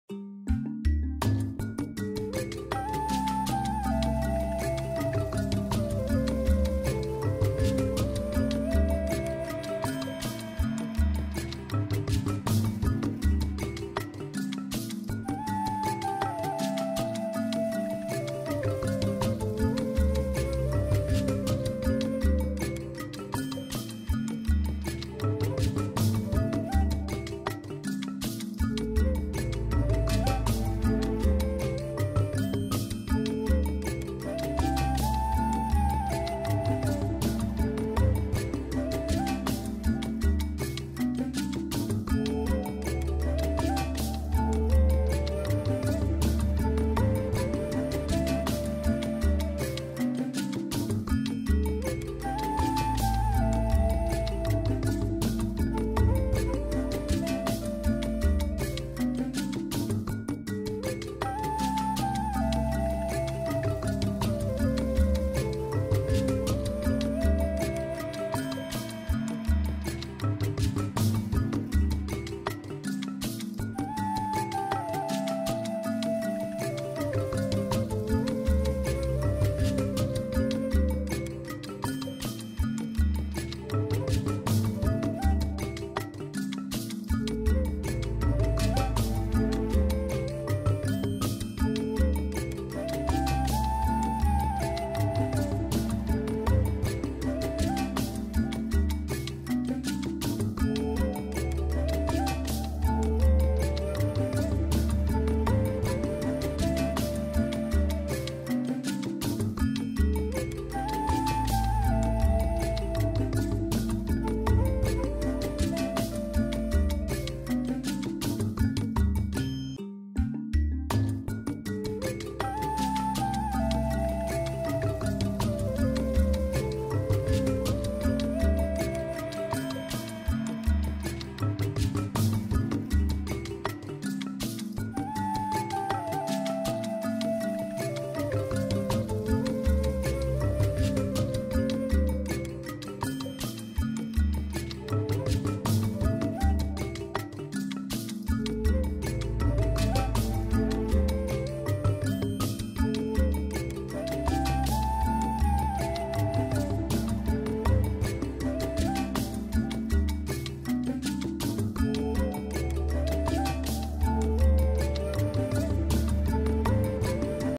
The Seawall and McGee Beach
Mwmeorial Day weekend.. People enjoy the beach amid COVID19 cautious reopening. Corpus Christi,  Texas